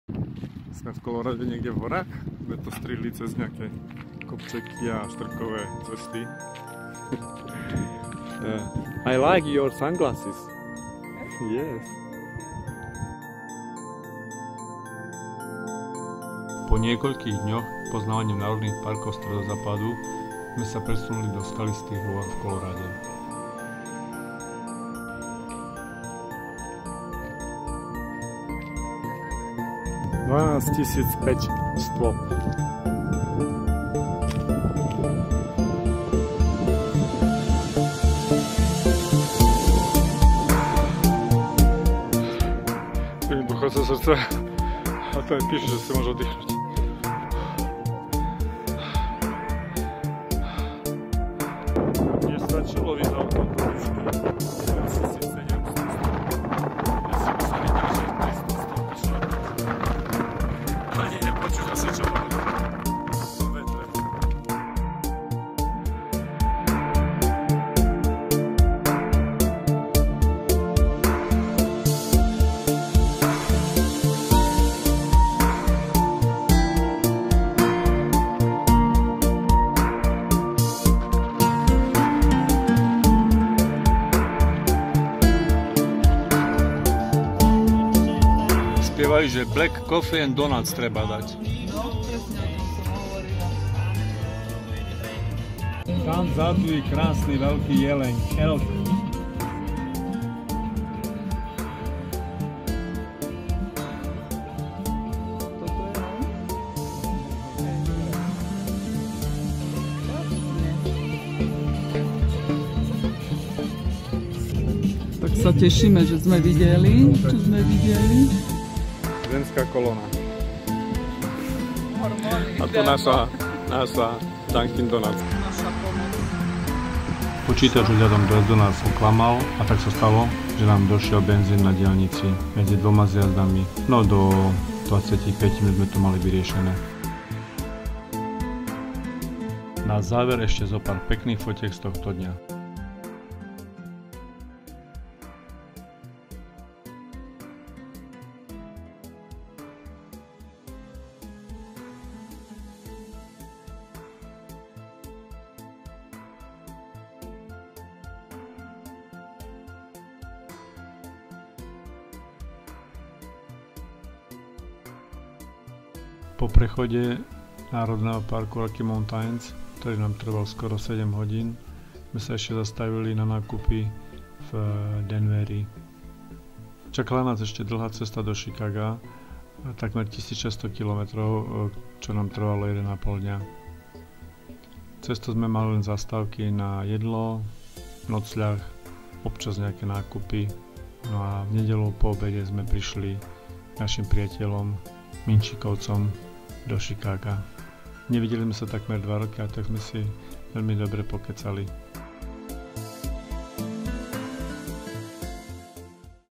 2019 USA 10-11.deň - NP Rocky Mountain a presun cez Nebrasku
Poznávacia dovolenka - vo dvojici bez opice - veľký okruh po národných a štátnych parkoch USA :)  - desiaty deň - ráno sladko vyspatí v hotelovej posteli sme vyrazili smer NP Rocky Mountain, ktorý je plný nádherných scenérií, skalných útvarov a úžasných ciest. Pomaly sme autom prešli cez hlavný hrebeň vo výške takmer 4000 m n.m. a popoludní sme pokračovali ďalej do Denveru. No a potom nás čakalo takmer 1600 km cez nekonečnú Nebrasku a Iowu až k priateľom do Chicaga.